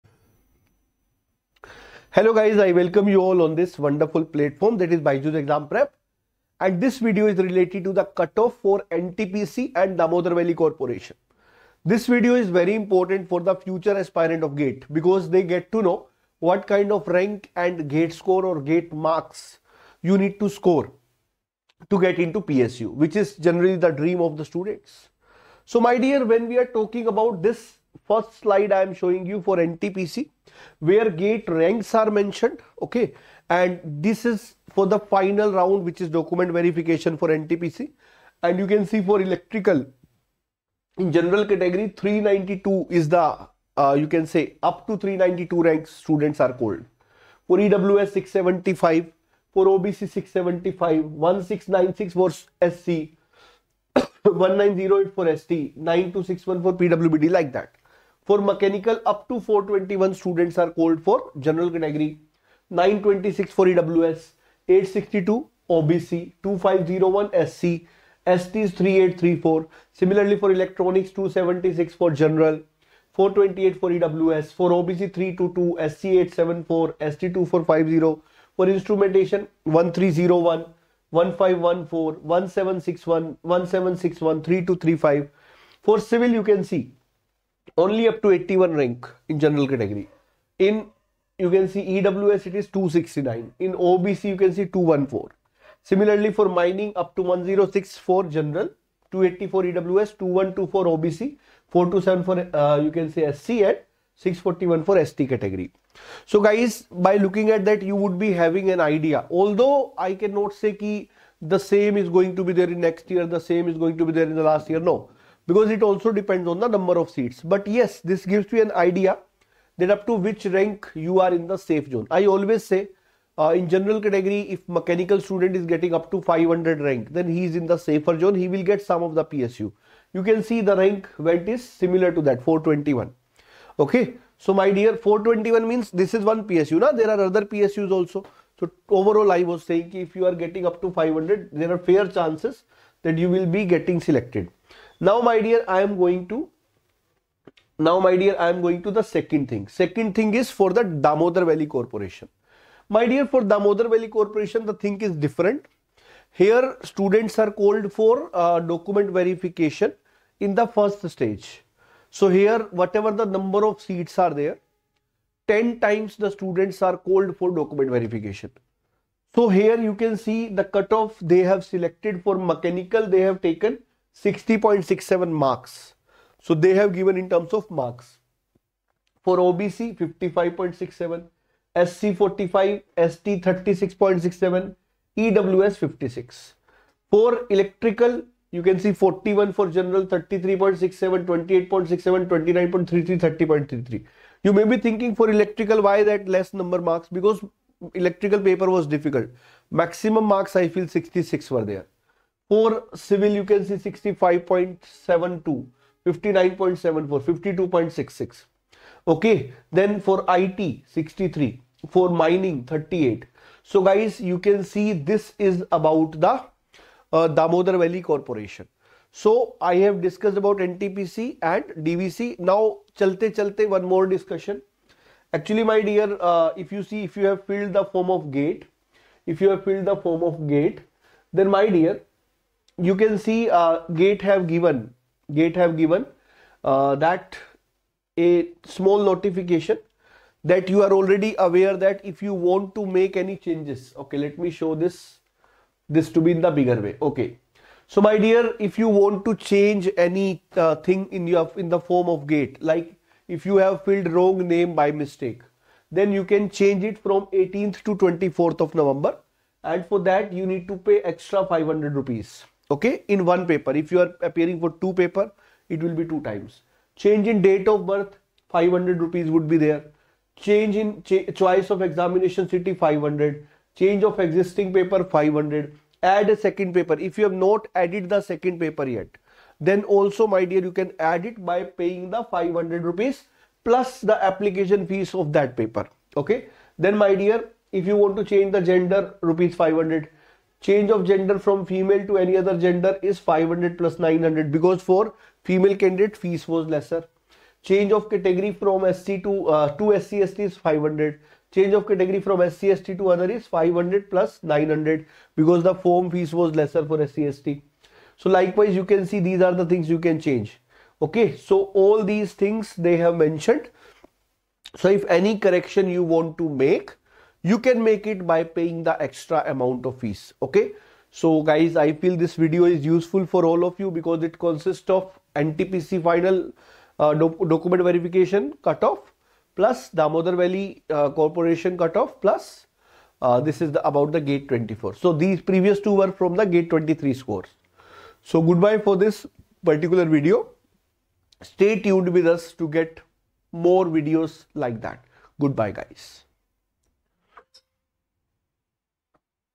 NTPC & Damodar Valley (DVC) | Cutoff Through GATE and GATE 2024 Important Update | BYJU'S GATE
📌📌 To Get Daily Practice Quizzes, Free Mock Tests, Subjectwise Questions

All India Open Mega Mock Test for GATE 2024